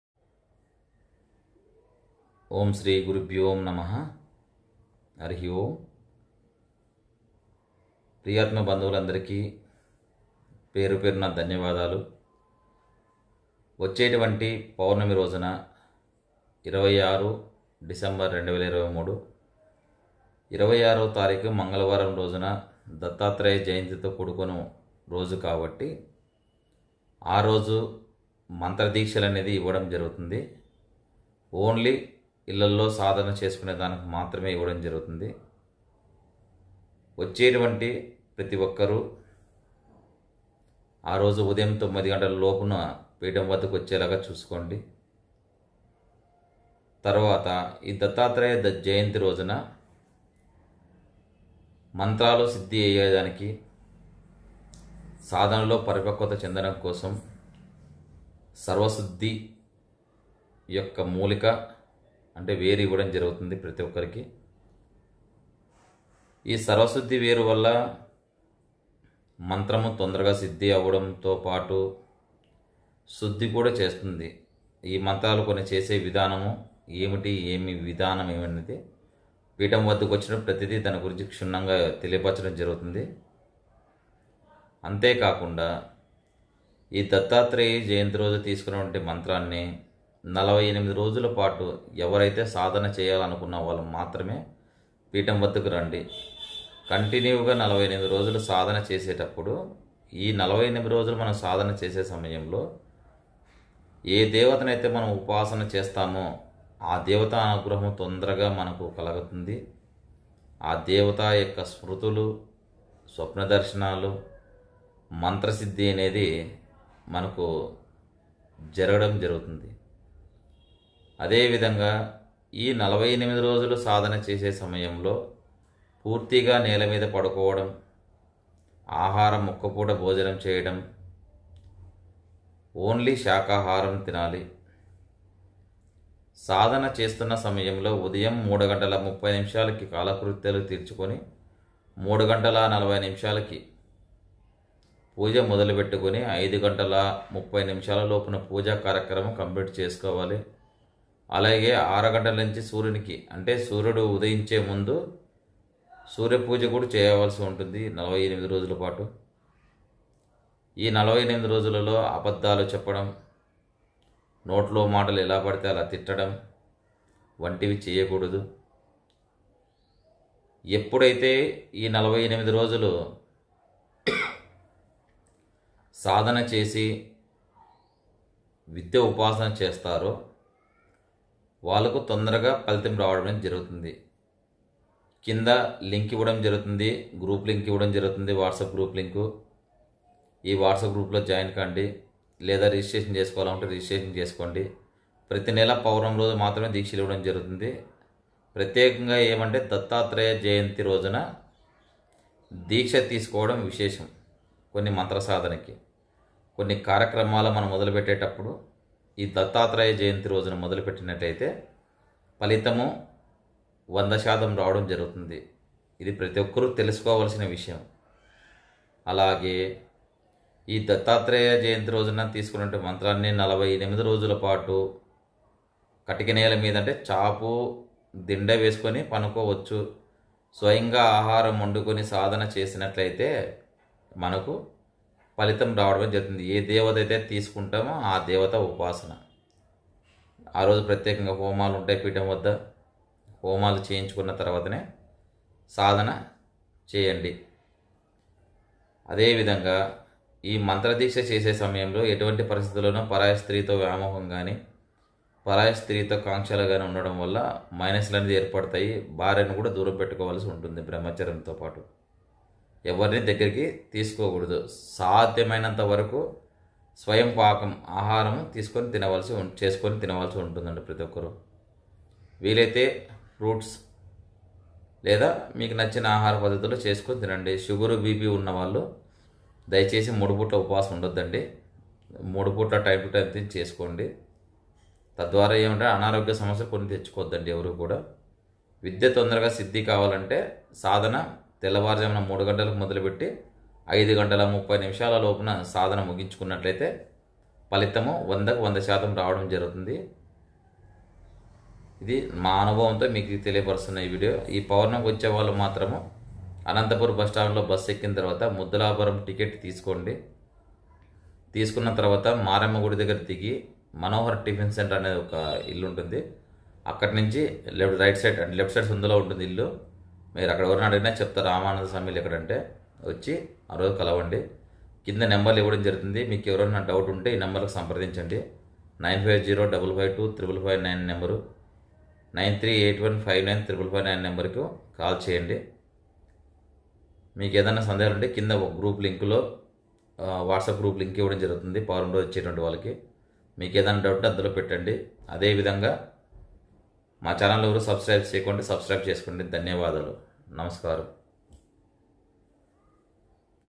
Unveiling the Power of Mantra Deeksha: Discover the Essence of Dattha Jayanthi srivajreswaripeetam
In this video, I'm unveiling the power of Deeksha - the essence of Dattha Jayanthi. Deeksha is the most powerful form of meditation and it can help you connect with your spiritual essence.

If you're looking for ways to connect with your spiritual energy, Deeksha is the meditation technique for you. This video will show you how to do Deeksha and how it can help you achieve your goals. After watching this video, you'll be able to use Deeksha to improve your health, increase your spiritual awareness and more!
Unveiling the Power of Deeksha: Discover the Essence of Dattha Jayanthi

dattajayanthi,datta sadhana,dattachi aarti,datta jayanti,dattajayanthi 2024,black magic,srivajreswaripeetam